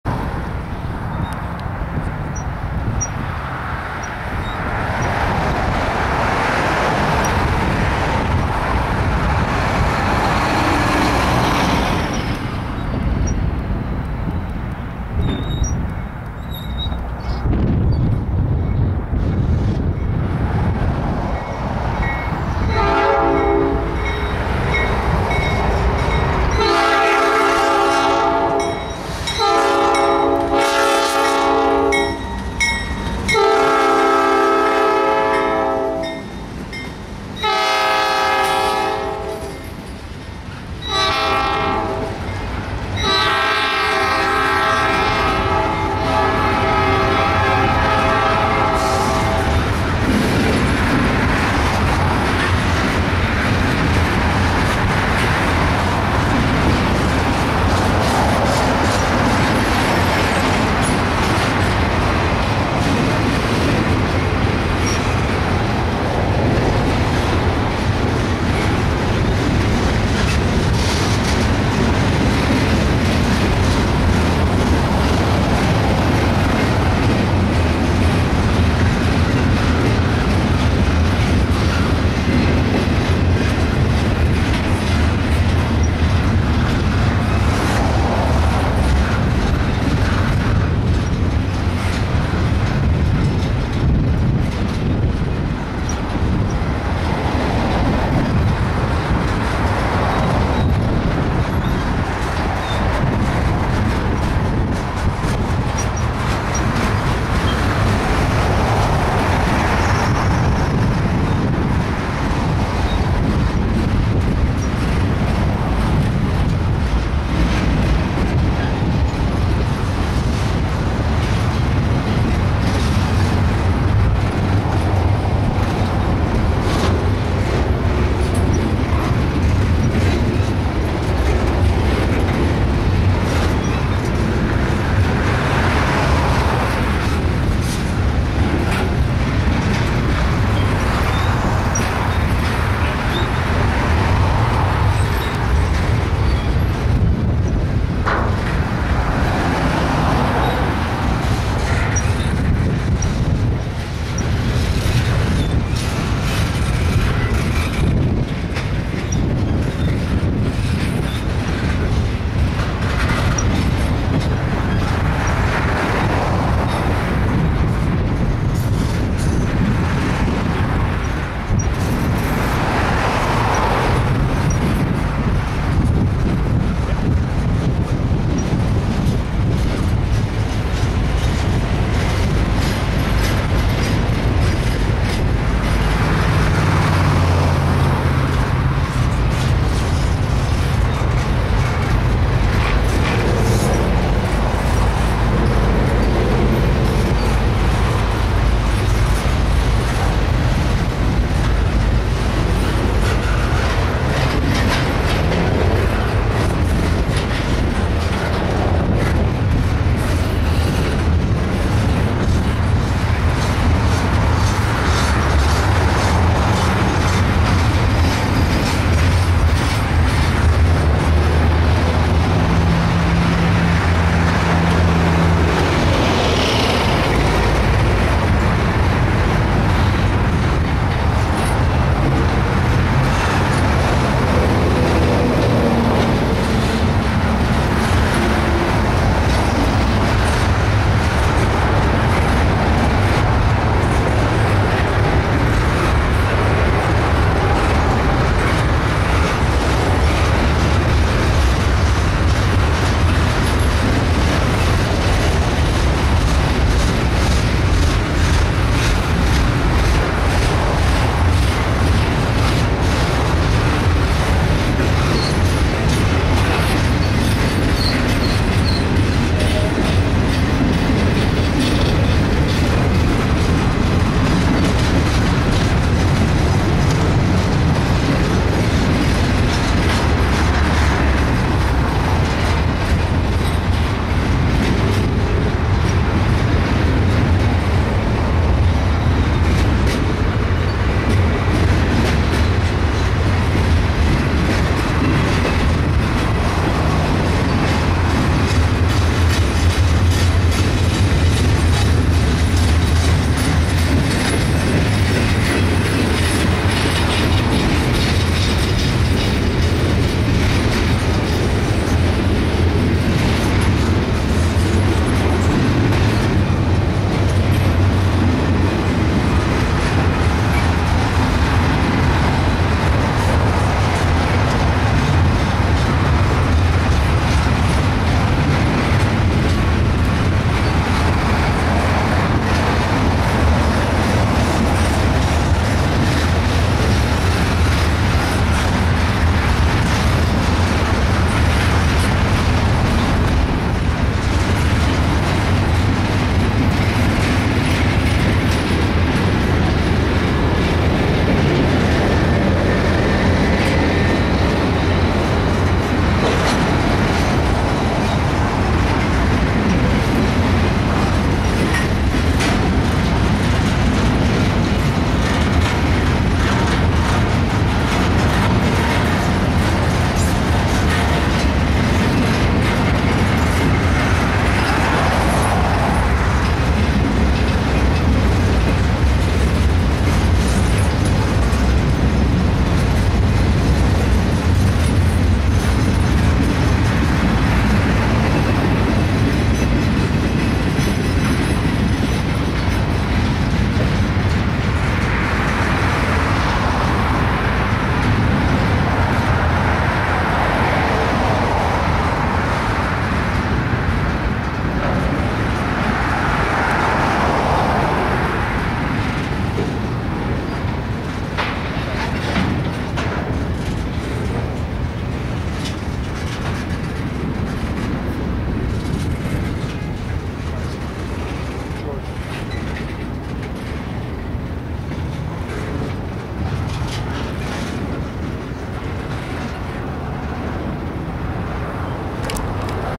Norfolk Southern northbound stack train in Austell,GA
A 142 car long Norfolk Southern stack train prepares to enter Whitaker Yard in Austell,GA at 3:19pm on Monday, 12/27/10.